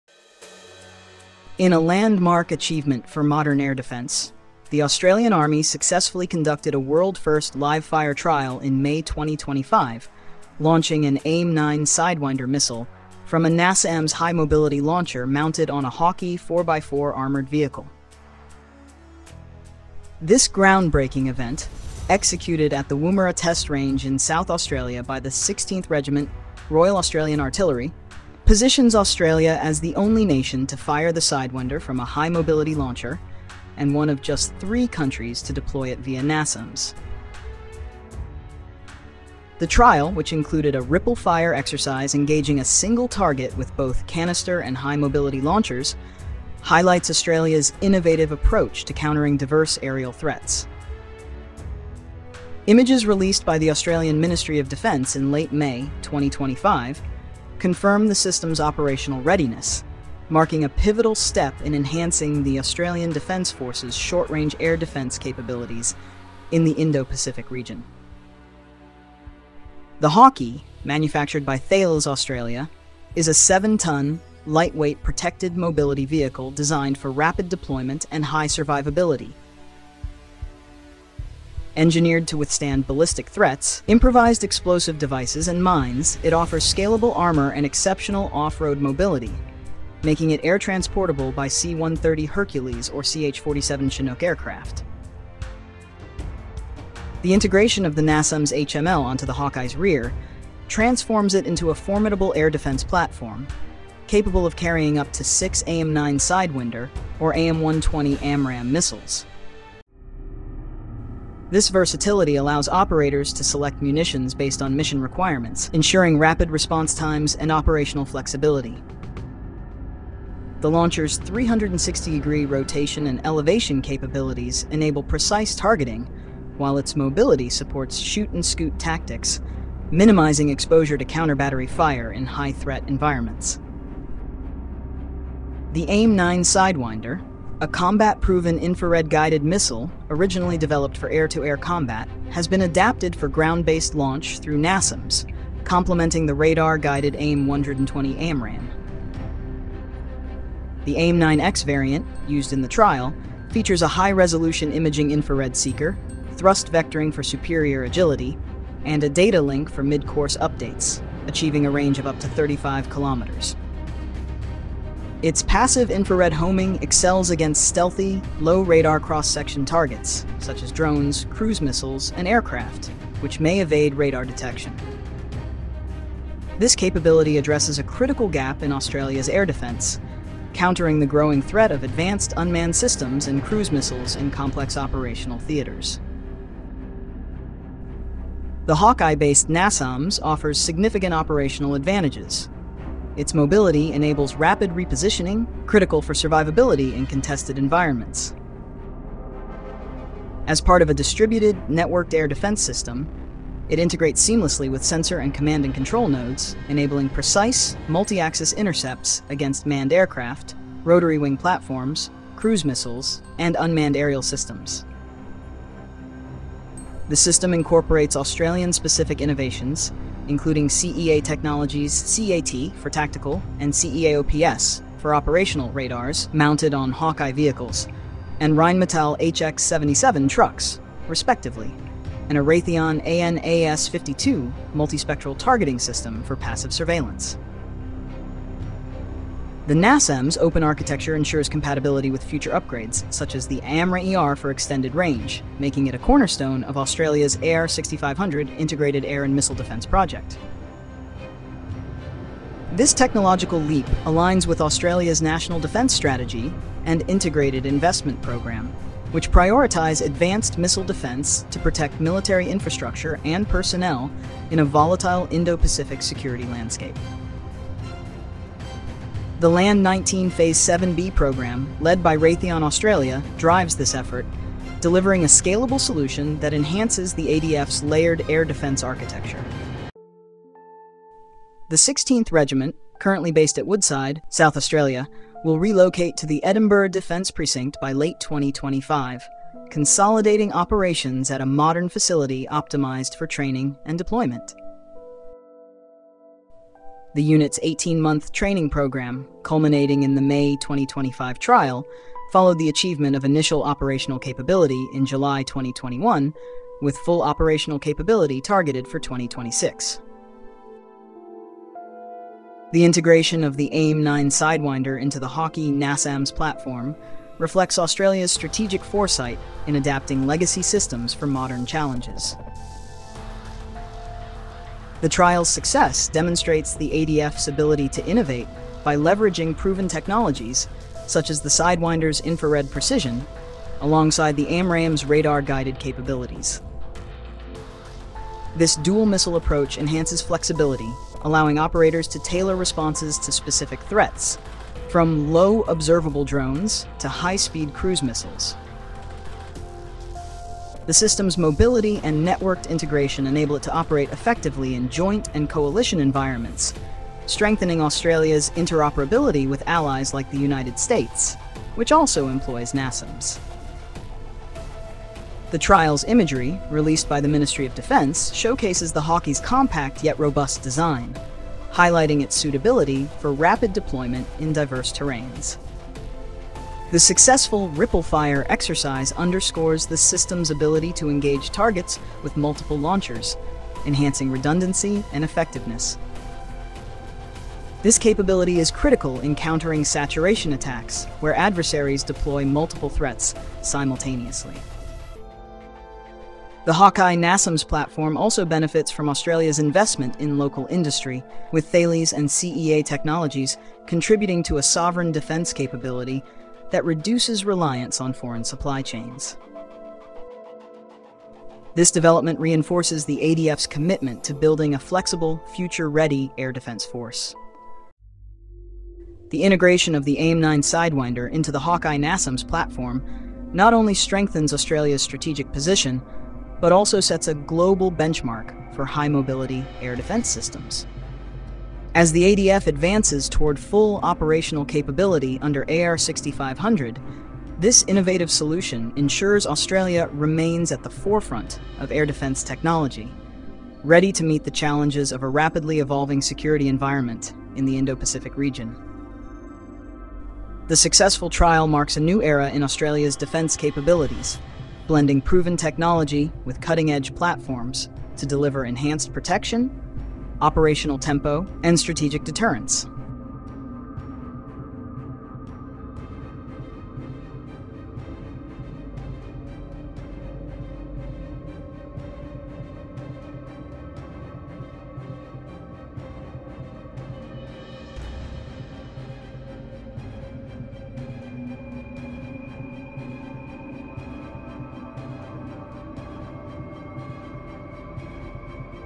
AIM-9 Sidewinder on Wheels: Australia’s Hawkei Just Rewrote Air Defense Rules!
In a world-first military breakthrough, the Australian Army has successfully fired an AIM-9 Sidewinder missile from a NASAMS High Mobility Launcher (HML) mounted on a Hawkei 4x4—proving that cutting-edge air defense no longer needs to be tied to heavy, static systems. This historic live-fire test, conducted at Woomera Test Range in May 2025 by the 16th Regiment, Royal Australian Artillery, marks a massive leap in mobile air warfare, positioning Australia as the only nation to launch a Sidewinder from a high-mobility platform and one of just three countries using it with NASAMS.

🔥 Key Highlights:
✔ First-Ever Hawkei + Sidewinder Launch – Watch as Australia debuts this game-changing combo.
✔ Ripple Fire Test – Dual launches from canister and HML launchers to overwhelm a single target.
✔ Stealth Killer – The AIM-9X’s infrared homing makes it deadly against drones, cruise missiles, and low-observable threats.
✔ Shoot-and-Scoot Tactics – The Hawkei’s speed and agility allow rapid repositioning to evade counterfire.
✔ Australian-Made Defense – Featuring CEA Technologies radars and Thales Hawkei’s battle-tested survivability.

🇦🇺 Why This Matters:
Australia’s new Hawkei NASAMS isn’t just a tech flex—it’s a strategic game-changer for the Indo-Pacific, where fast-moving aerial threats demand lightweight, mobile air defense. With 360° targeting, six-missile capacity, and C-130 air transportability, this system ensures the ADF can protect forward bases, troops, and critical assets like never before.

🔍 Inside the Tech:
    AIM-9X Sidewinder – Infrared guidance, thrust vectoring, and 35km range.
    Hawkei 4x4 – 7-tonne, mine-resistant, and built for rapid deployment.
    NASAMS HML – Open-architecture system ready for future missiles like AMRAAM-ER.
    Networked Warfare – Integrated with CEATAC & CEAOPS radars for unmatched situational awareness.

📅 What’s Next?
With full operational capability expected by 2026, this system is a cornerstone of Australia’s AIR 6500 missile defense program—ensuring the ADF stays ahead in an era of drones, hypersonics, and stealth threats.

💬 What do you think? Could this be the future of mobile air defense?